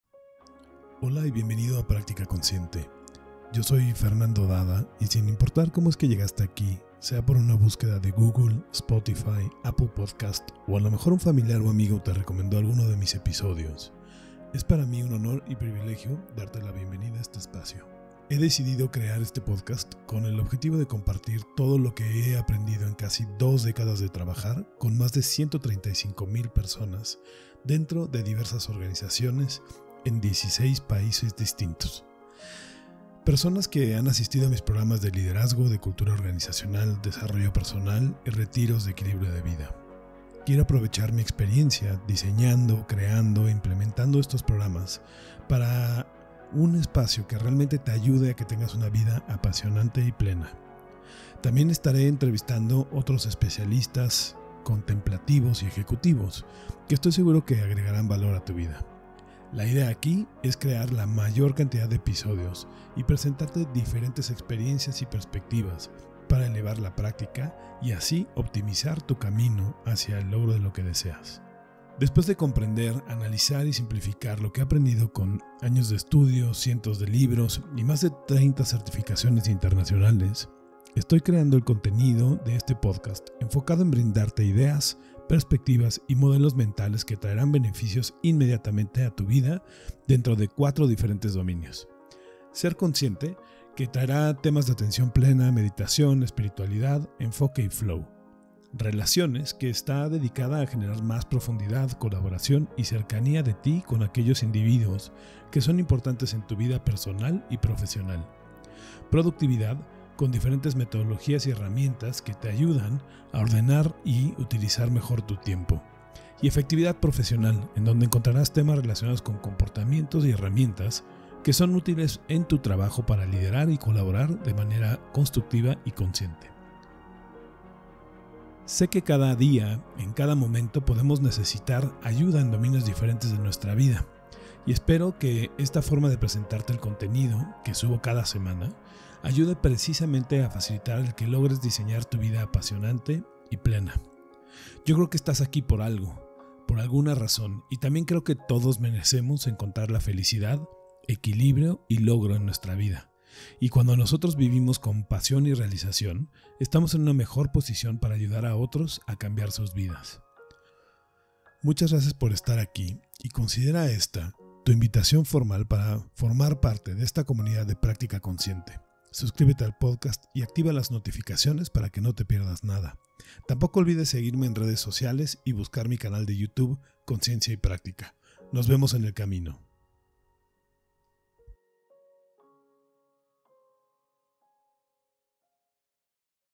001: Bienvenidos a Práctica Consciente
Si quieres sabés desde dónde, porqué, para qué y qué es lo que este podcast te va a brindar, escucha este episodio.